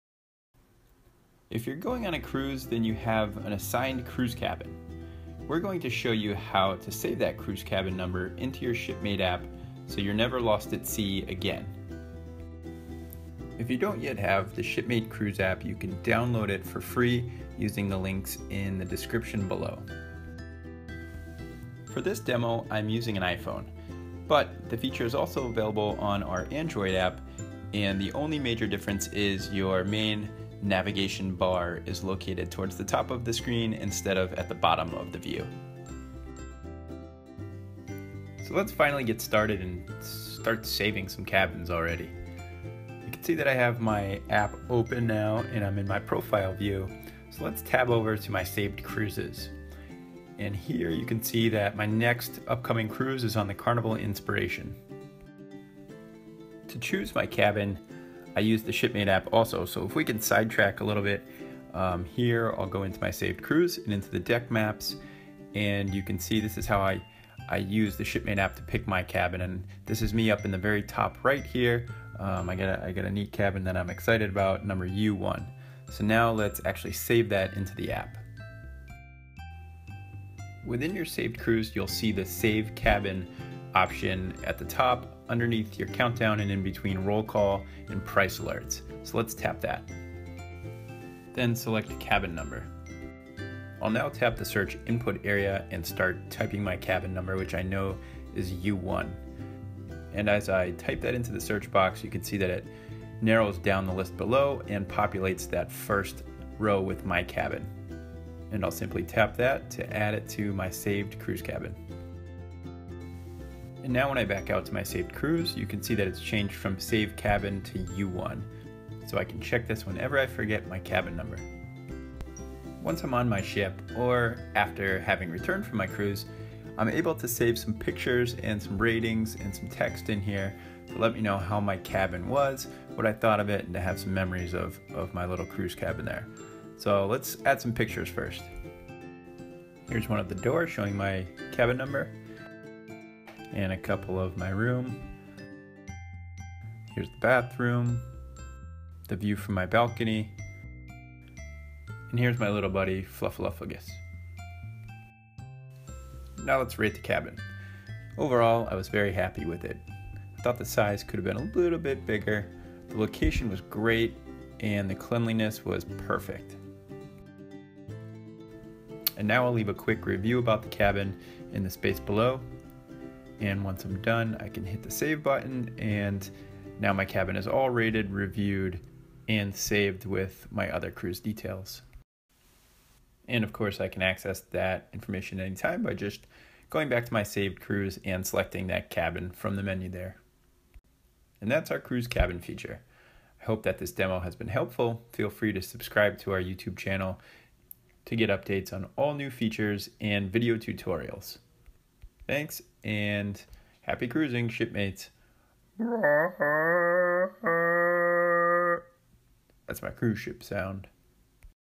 Ship Mate Tutorial #4: Cruise Cabin Feature
Cruise Cabin Feature on Ship Mate

This video demo will show you how to save your cruise cabin into Ship Mate, and to review your stateroom once you've set sail.

You can also see the full blog post with more instruction